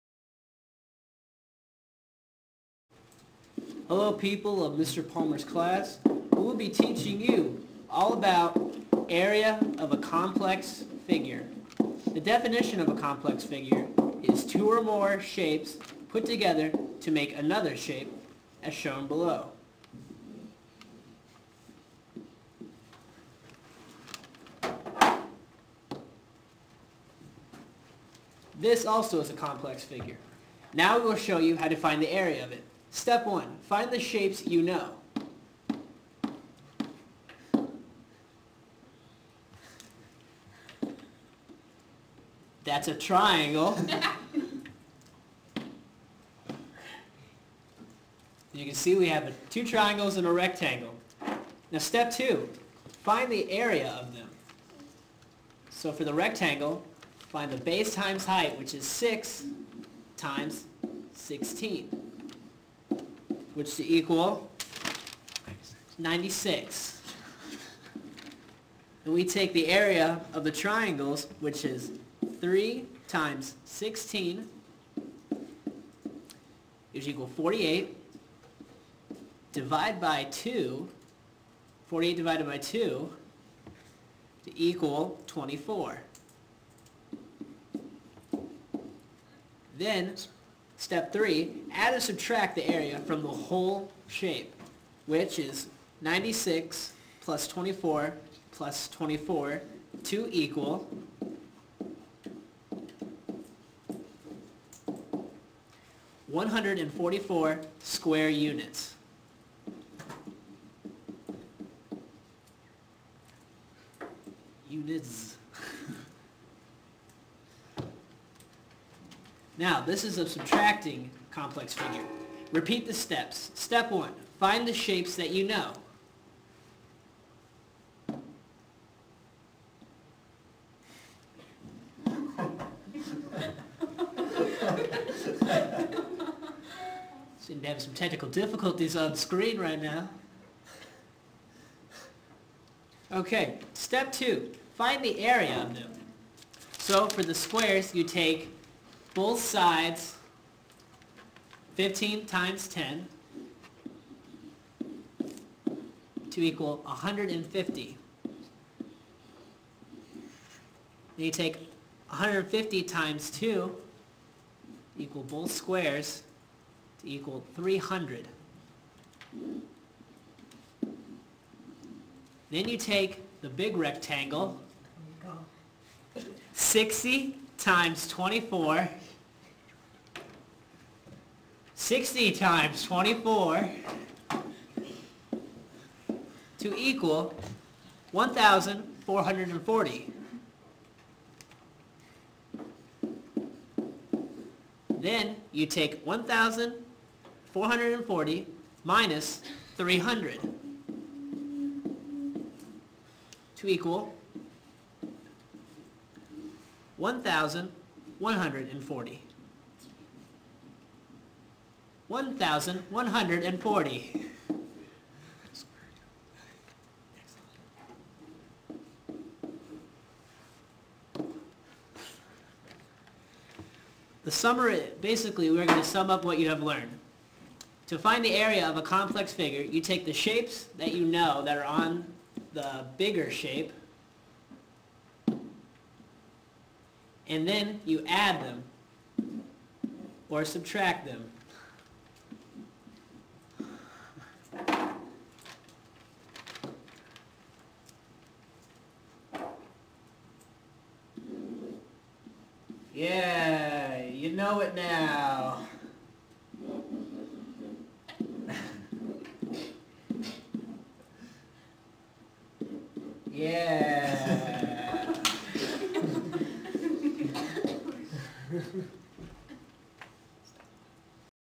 Area of complex figures 2
Another student made video on how to find the area of complex figures.